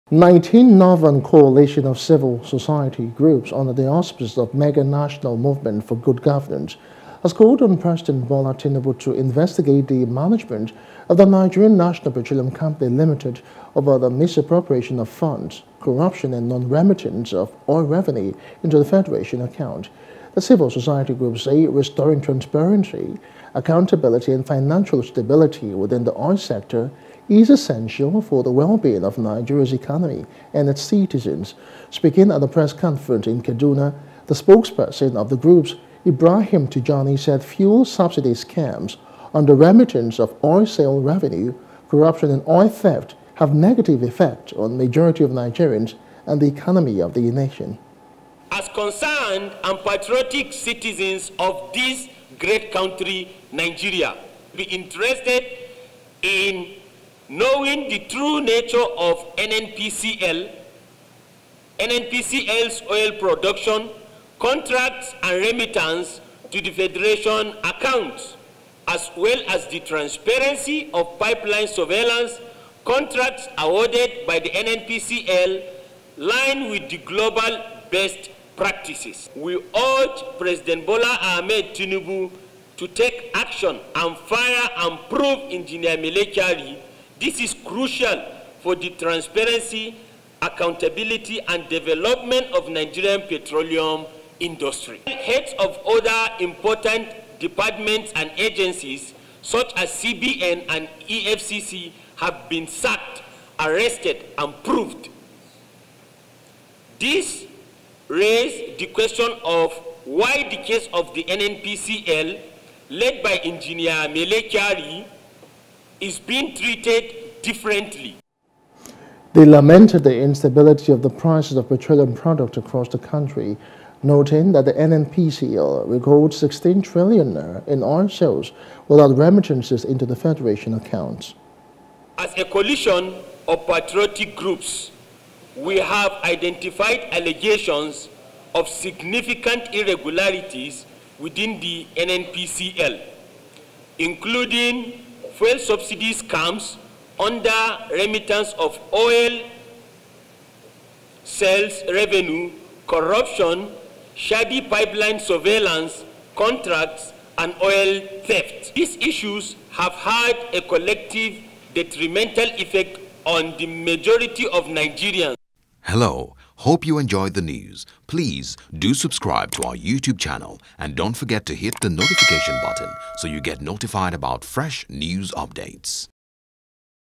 Alleged Corruption: Northern Groups Urge Tinubu To Restructure N.N.P.C.L
19 Northern Coalition of Civic Societies groups under the auspices Mega National Movement for Good Governance has called on President Bola Tinubu to investigate the management of the Nigerian National Petroleum Company over misappropriation of funds, corruption and non-remittance of oil Revenue into federation account.